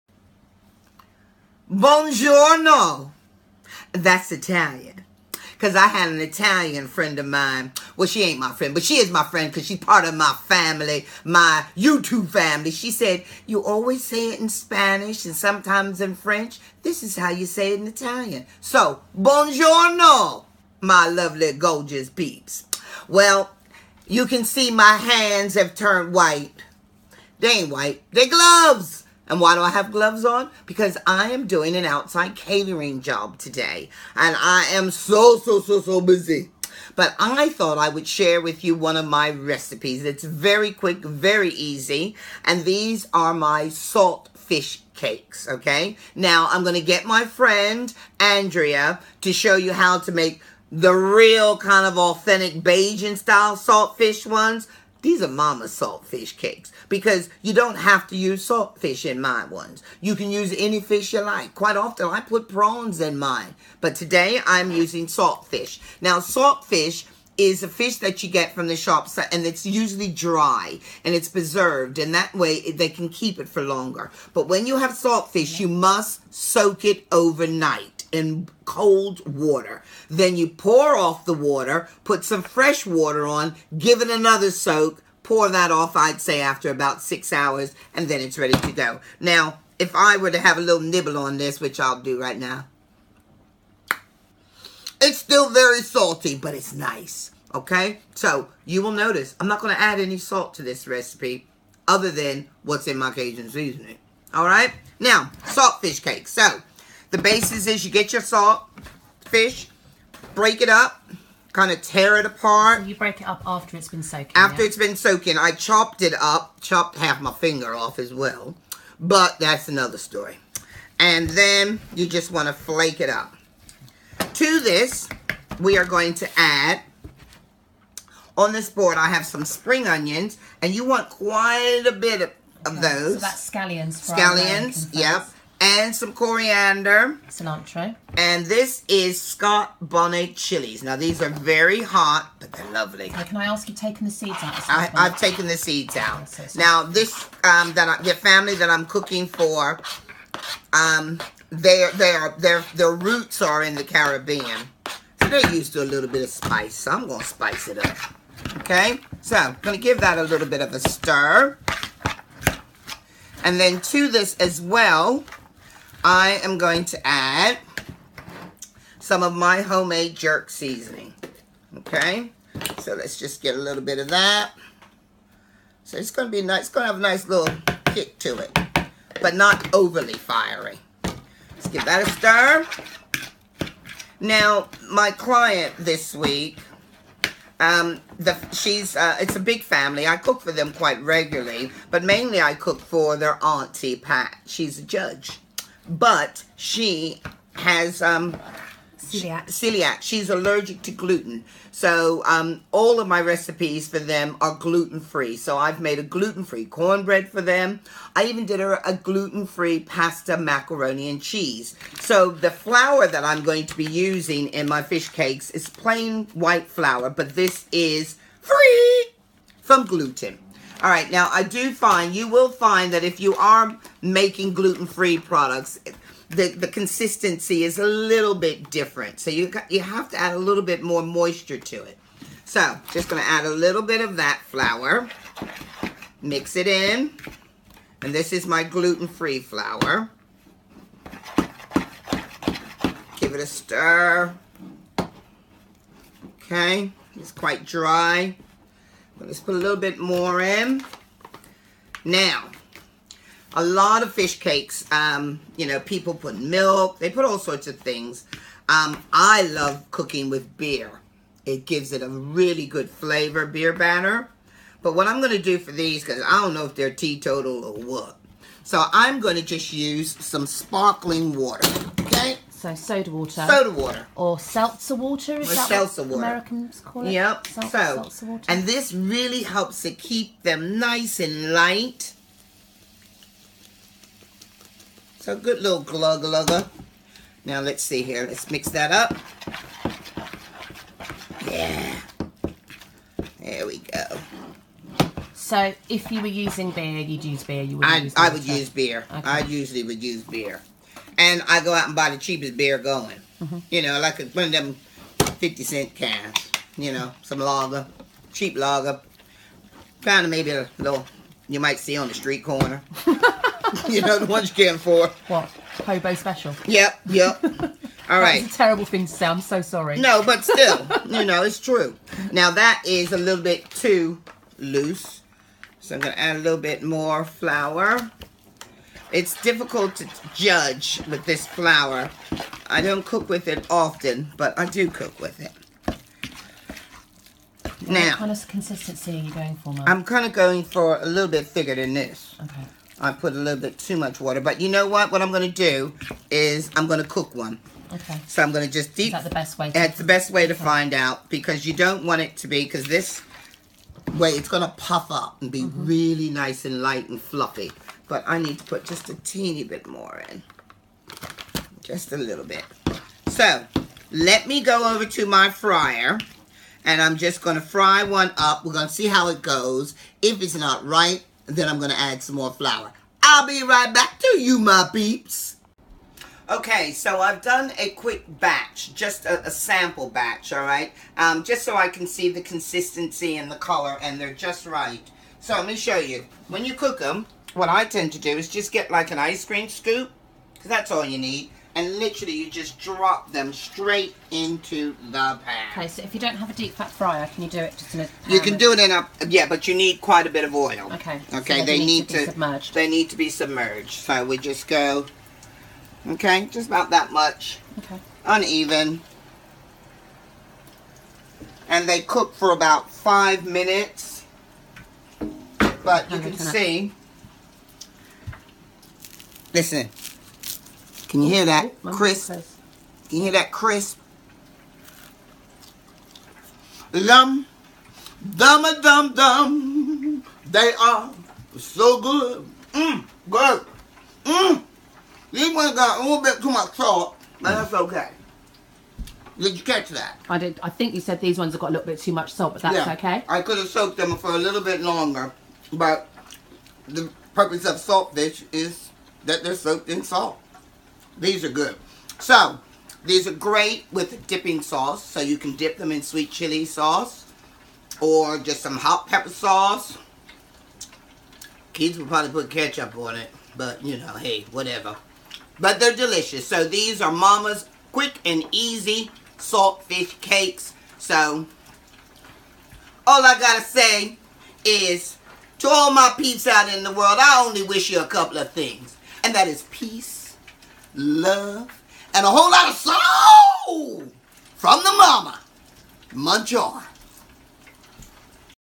How to make Salt fish cakes
Momma's version of Bajan Salt fish cakes. Quick, easy, and delicious. Full recipe available on our website mommacherrisoulfood.com